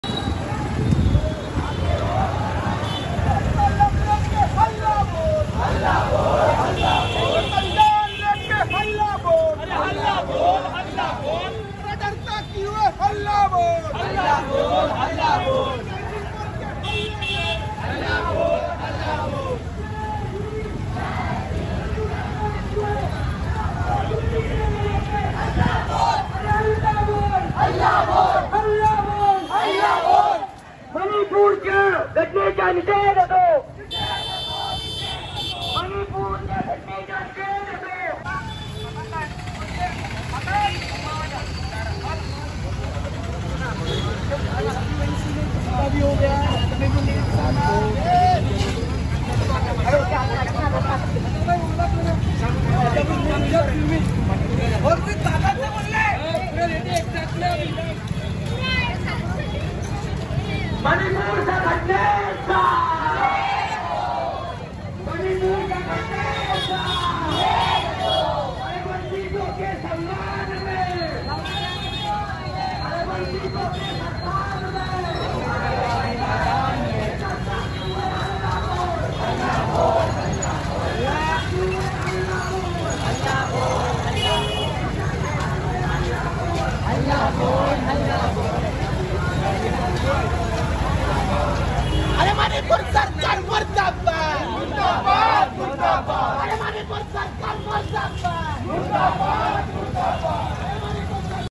RAISING VOICES FOR WOMEN'S SAFETY IN MANIPUR
Aadiwasi Samajat Jhaleleya aateyachar virodhat nagpuratil jagpurk yuvkancha Shantipurvak candle March